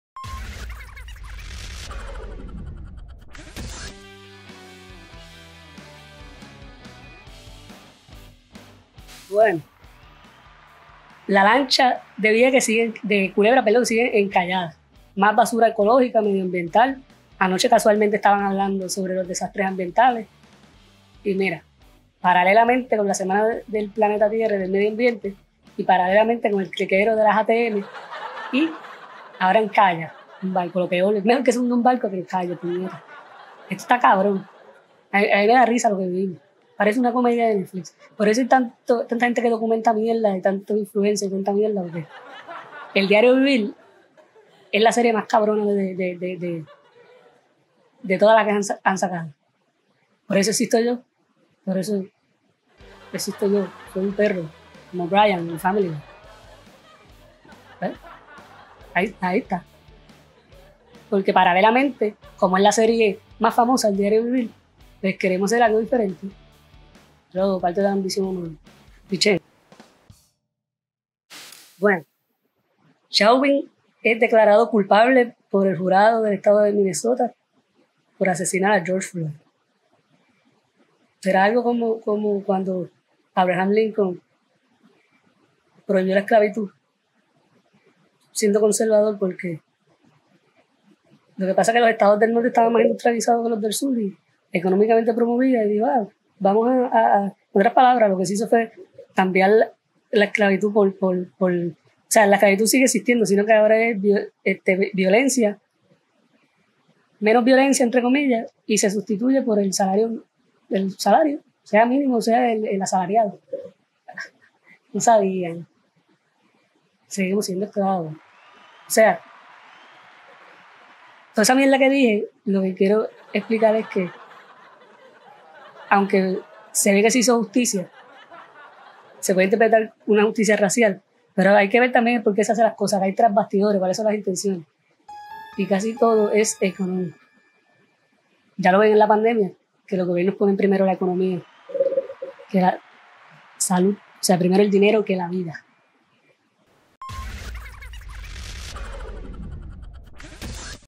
¿George Floyd Justicia Real?
Bote encalla en Culebra. Siguen las ATM y lanchas siendo el asme reír del gobierno de Puerto Rico.
¿Realmente existió  justicia racial sincera en USA por caso de George Floyd?

Instagram: @naughtypopko Twitter: @naughtypopko
Spotify: @naughtypopko Apple Podcast: @naughtypopko